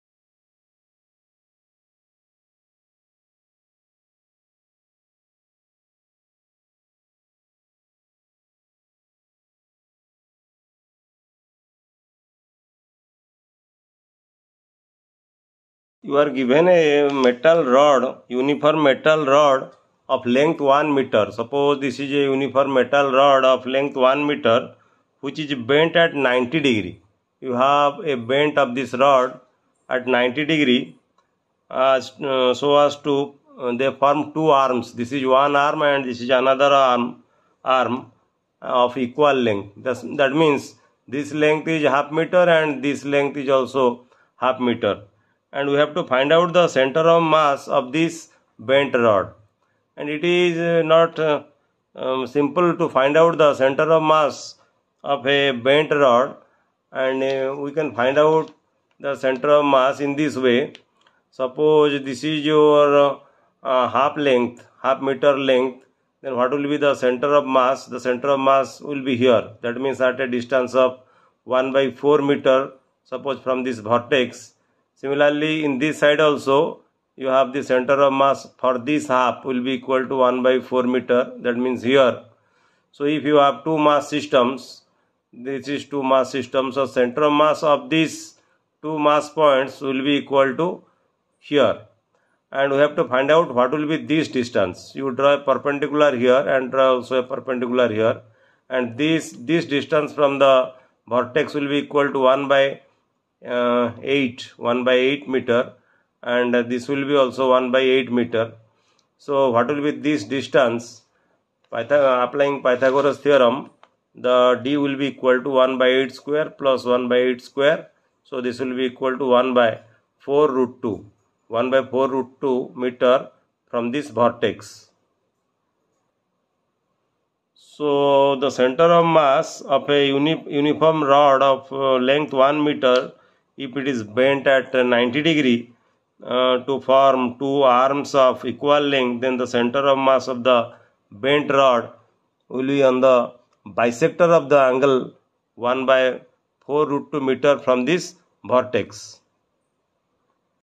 A uniform metal rod of length 1m is bent at 90deg. to form two arms of equal length. Find CM
A uniform metal rod of length 1m is bent at 90deg. to form two arms of equal length. Find CM @PTE